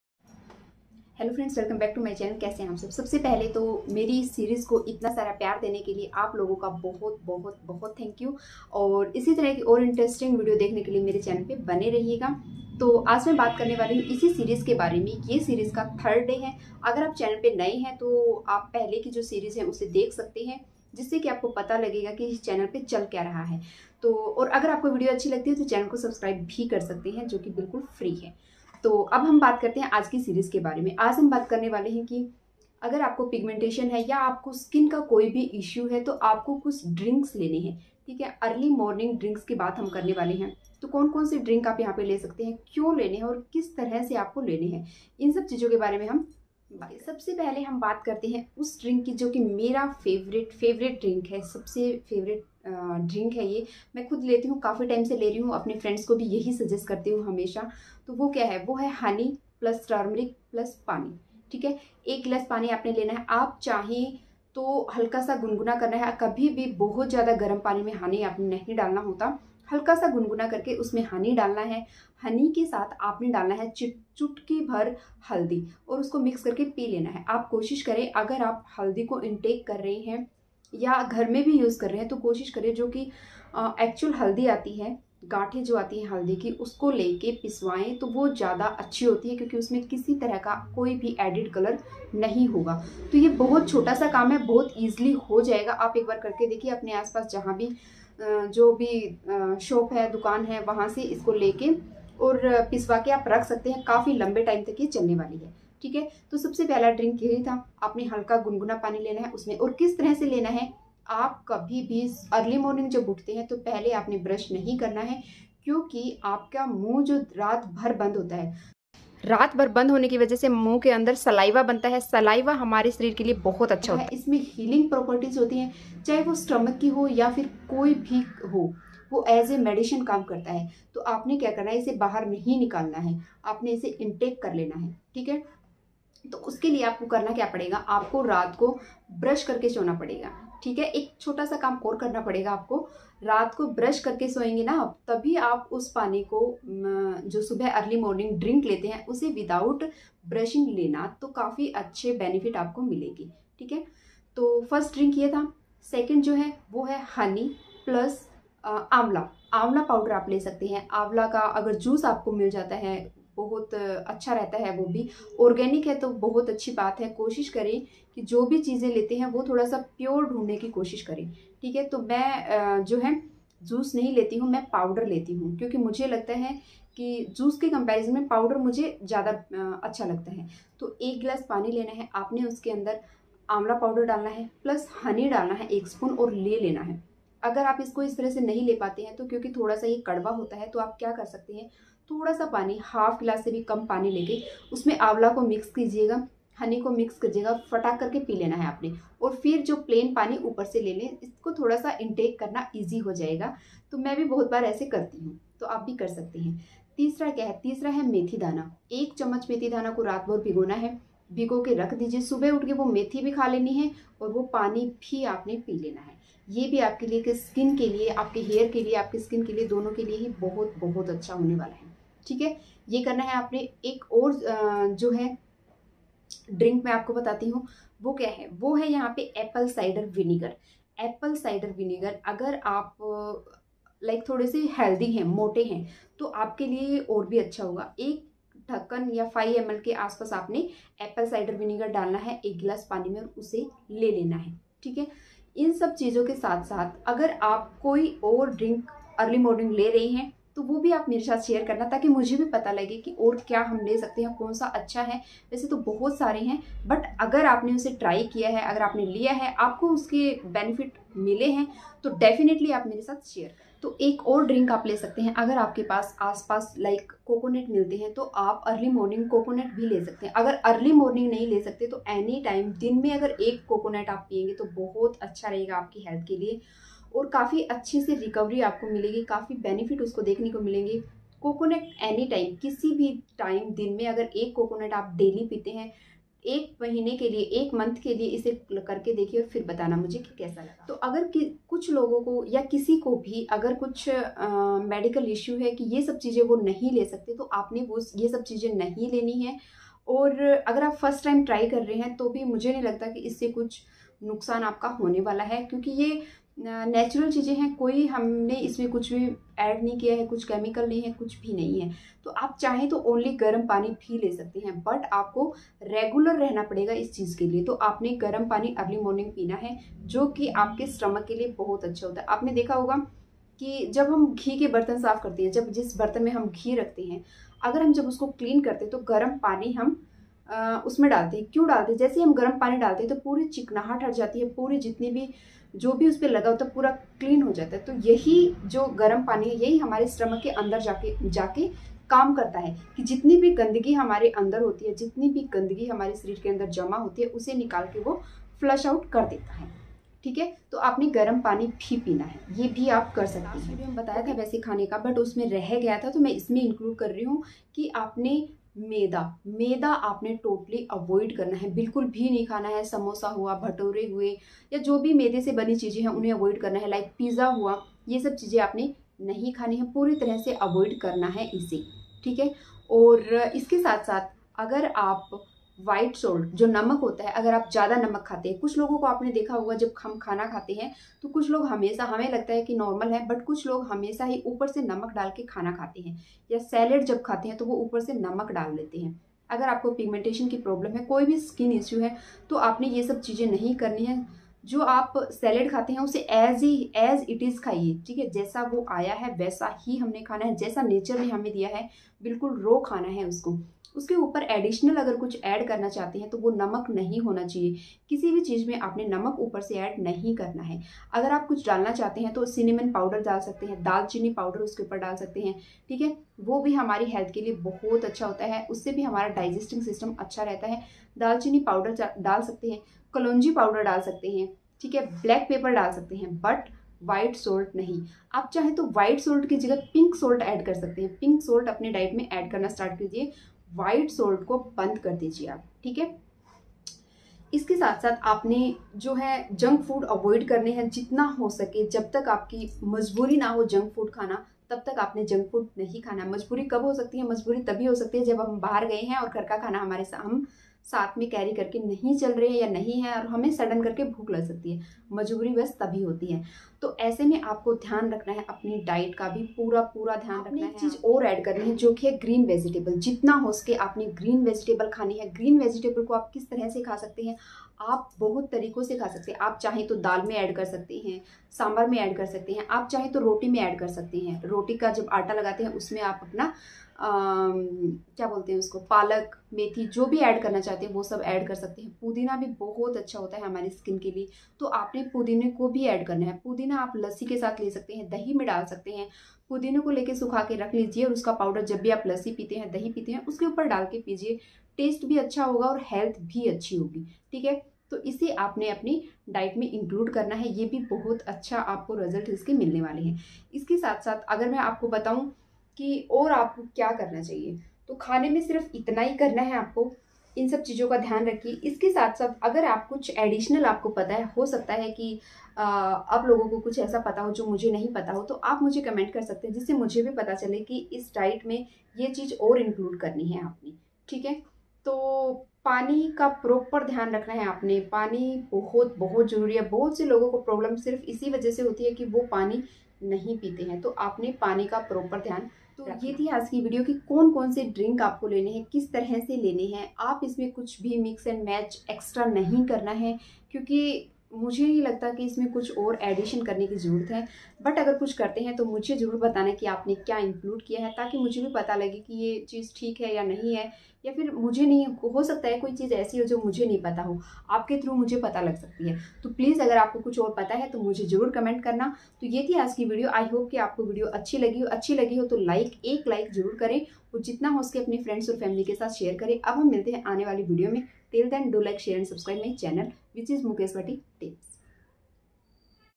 Series Step :3 Improve Your Skin With Some Powerful Drinks |Very Important Morning Drinks & More
Morning drink for Pigmentation Series Step:3 For Your झाइयां  Remove  जड़ से ll

👉 Hello to my beautiful family 🥰

        🥰Mukesh Bhatti tips 😊🙏

👉 In this video I am sharing some healthy morning drinks for healthy skin ...

    👉 If you have Pigmentation you must
     follow...If you don't have pigmentation
     then also you can use this one for a
     glass and spots free skin..

     👉Pigmentation and Dark Spots Remove Series Step 3..झाइयां ठीक करने का 2nd  कदम ll

Follow kare jo bhi m share kr rhi hu or aap dekhoge ki apki skin improve ho rhi h... aapko lagatar ise follow krna h tabhi results milengey.
To aap bhi step by step mere sath ise star kre or ab m hr din aapse milne wali hu...☺️

👉If you are having so much active acne please don't do. Don't irritate your skin.

👉Video links

👉Face Massage